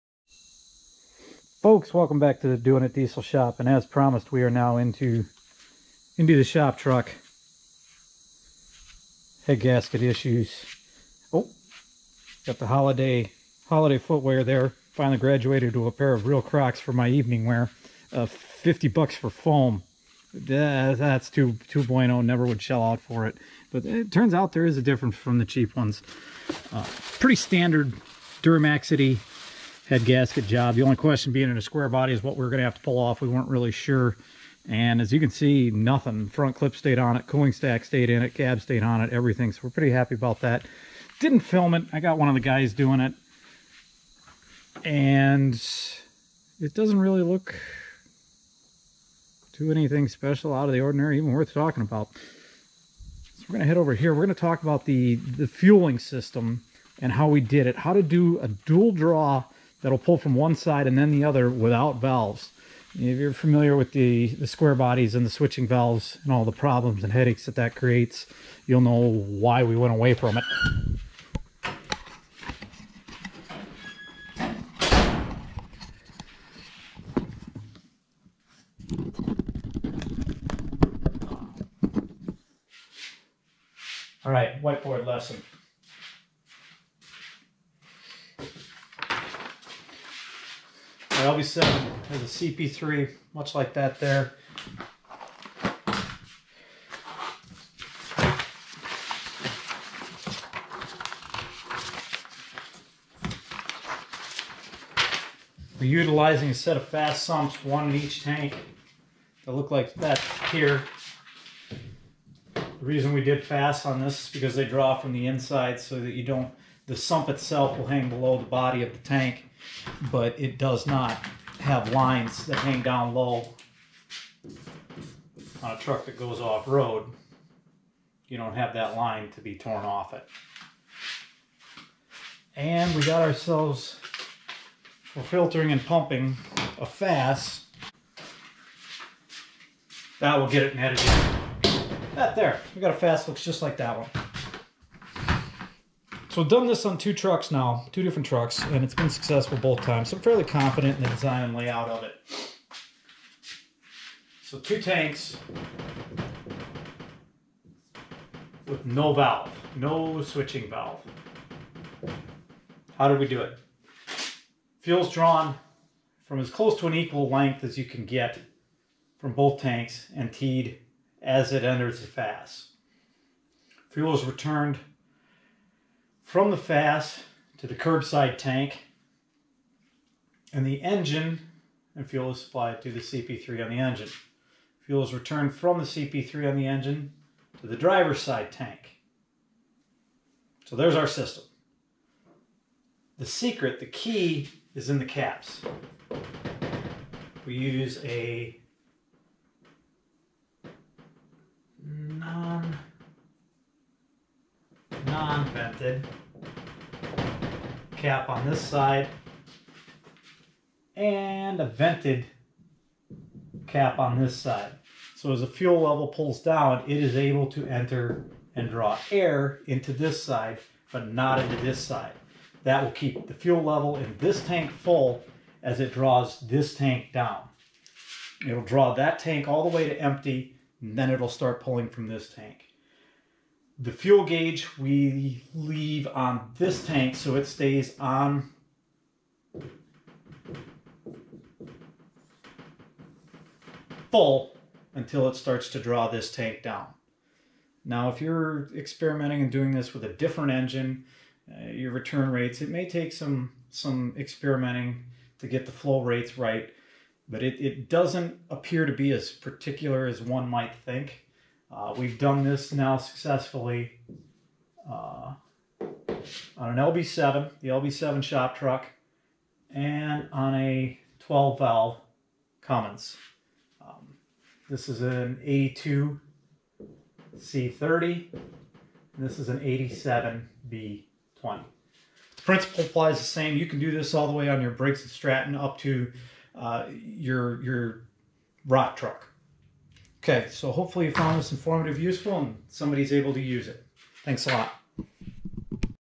Auto switching dual fuel tanks with no valve on the duramax squarebody
We take a quick peak under the hood and then dive into the fuel system design on the duramax squarebody shop truck. A dual tank system that switches tanks automatically and does it with no valve.